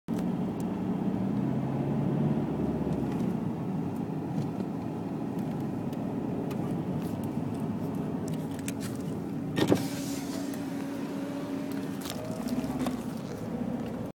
Lepre VS Alfa 147
Al mattino che mi tocca gareggiare con una lepre...